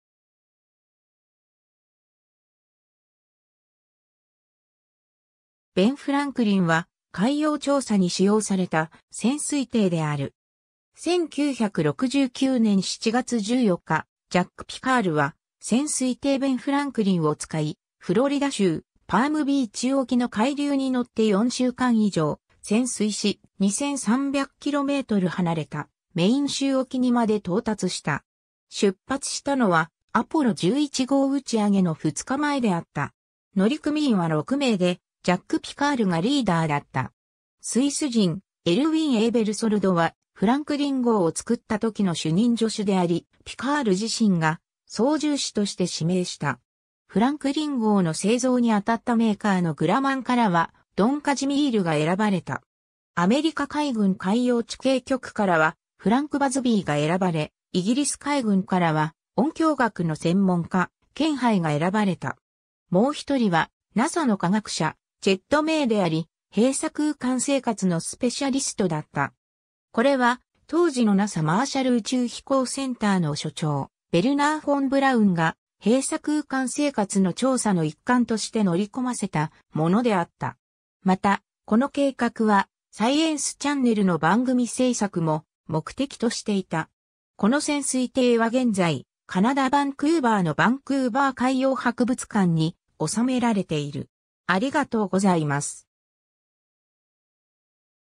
ベン・フランクリン (PX-15)
ベンジャミン・フランクリン
ノースロップ・グラマン
ベン・フランクリンは海洋調査に使用された潜水艇である。
 1969年7月14日、ジャック・ピカールは潜水艇ベン・フランクリンを使い、フロリダ州パームビーチ沖の海流に乗って4週間以上潜水し、2,300キロメートル離れたメイン州沖にまで到達した。
出発したのはアポロ11号打ち上げの2日前であった。
 乗組員は6名で、ジャック・ピカールがリーダーだった。
スイス人エルウィン・エーベルソルド（Erwin Aebersold）はフランクリン号を作った時の主任助手であり、ピカール自身が操縦士として指名した。
フランクリン号の製造にあたったメーカーのグラマンからはドン・カジミール（Don Kazimir）が選ばれた。
アメリカ海軍海洋地形局（Oceanographic Office）からはフランク・バズビー（Frank Busby）が選ばれ、イギリス海軍からは音響学の専門家、ケン・ハイ（Ken Haigh）が選ばれた。
もう一人はNASAの科学者チェット・メイ（Chet May）であり、閉鎖空間生活のスペシャリストだった。
これは、当時のNASAマーシャル宇宙飛行センターの所長ヴェルナー・フォン・ブラウンが、閉鎖空間生活の調査の一環として乗り込ませたものであった。
また、この計画は、サイエンスチャンネル (Science Channel) の番組制作も目的としていた。
この潜水艇は現在、カナダバンクーバーのバンクーバー海洋博物館に収められている。